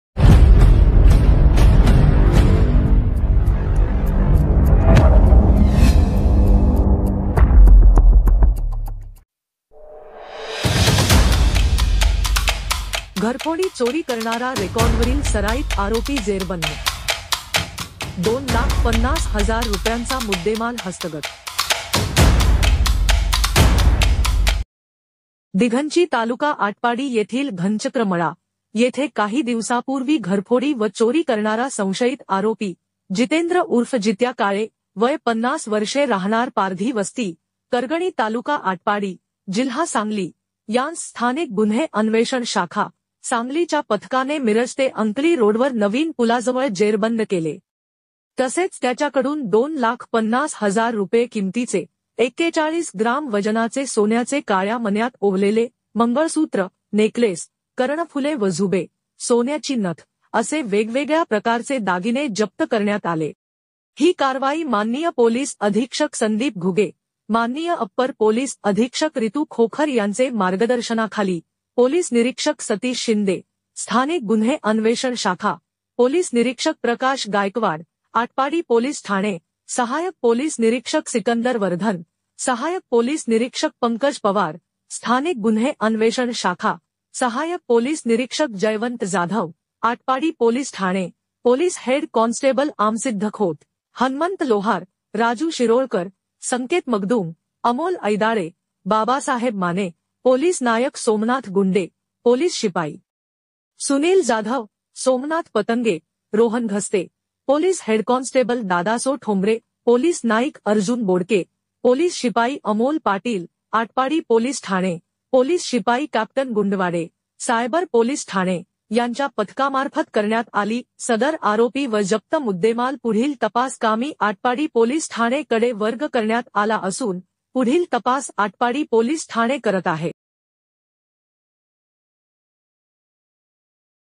घरफोडी चोरी करणारा रेकॉर्डवरील सराईत आरोपी जेरबंद,2 लाख 50 हजार रुपयांचा मुद्देमाल हस्तगत.
घरफोडी चोरी करणारा रेकॉर्डवरील सराईत आरोपी जेरबंद,
2 लाख 50 हजार रुपयांचा मुद्देमाल हस्तगत.